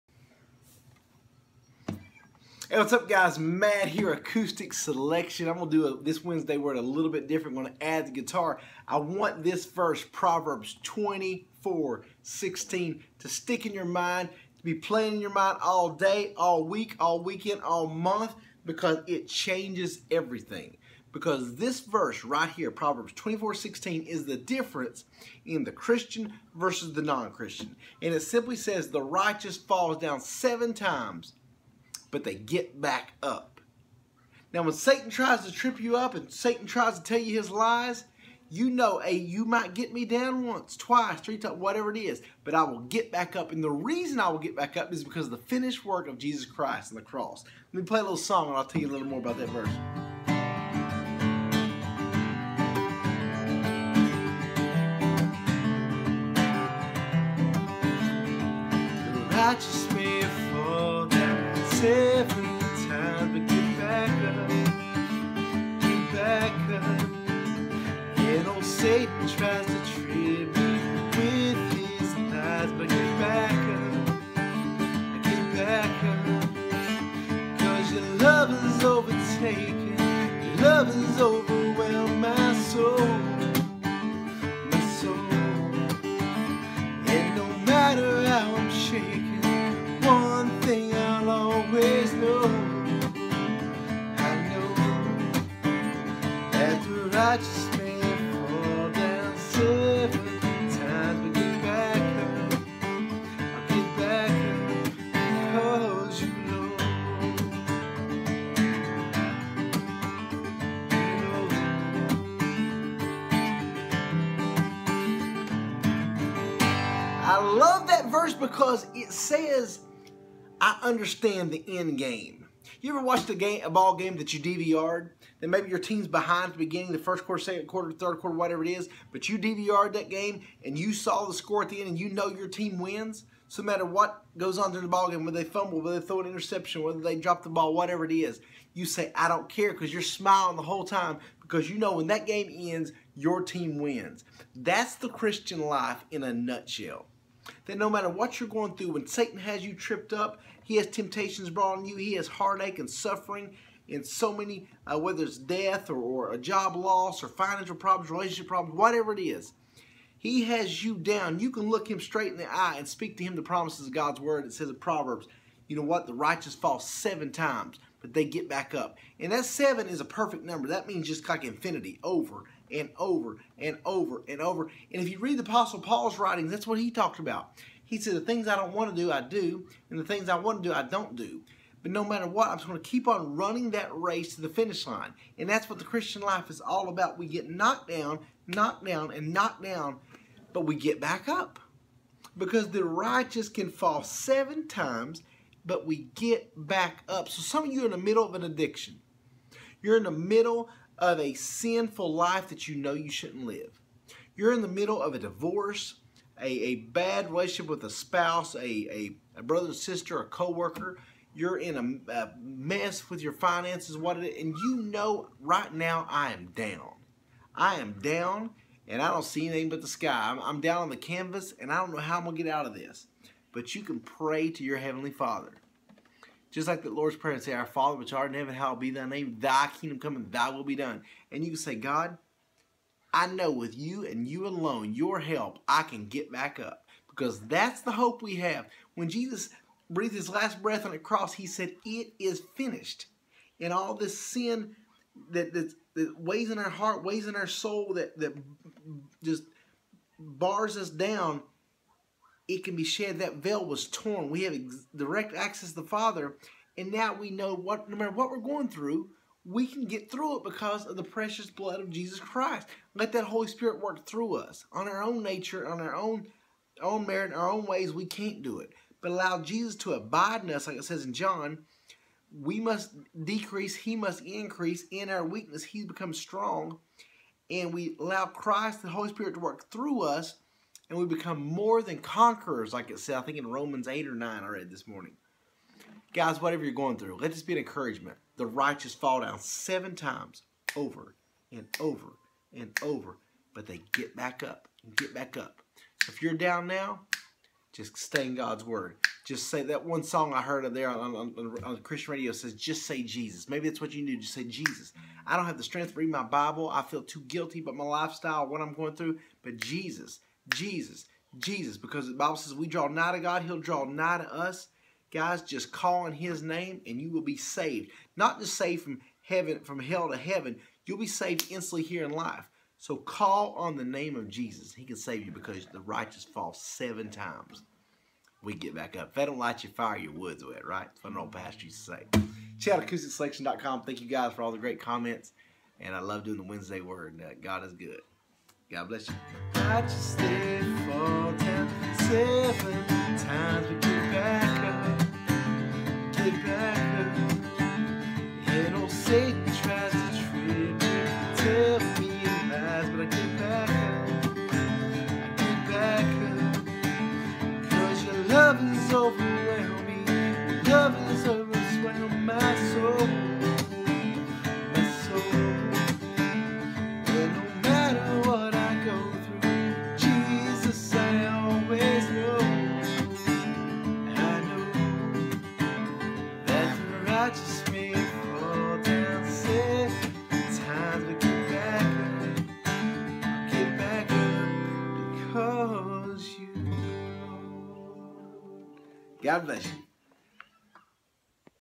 Get Back Up! | Wednesday Word | Matt McCoy
The biggest difference in a christian vs the non christian is we have a hope and future. Check out this week's wednesday word and leave here encouraged for what God has in store.
God bless!
Matt